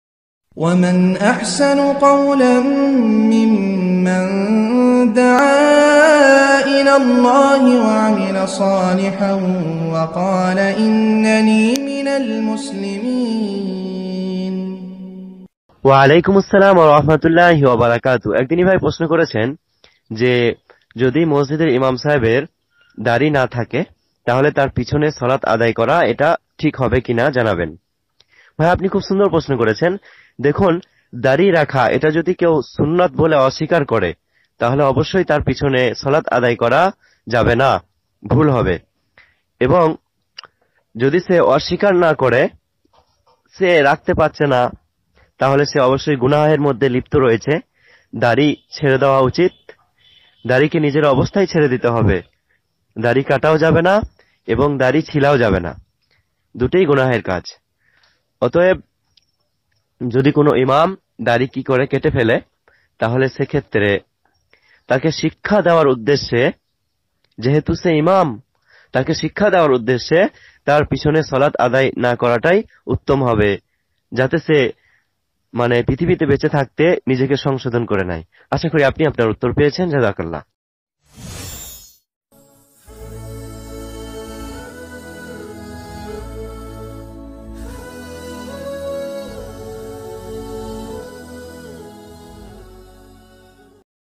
দাঁড়ি রাখেনা এমন ইমামর পেছনে স্বলাত আদায় করা যাবে কি ? শাইখ সাকিব আল বুখারী বর্ধমানী
দাঁড়ি রাখেনা এমন ইমামর পেছনে স্বলাত আদায় করা যাবে কি ?

🎤 শাইখ সাকিব বিন আব্দুল হাকীম আল বুখারী বর্ধমানী।

🎤 Answer / Shaikh Muhammad Sakib Al-Bukhari Burdwani.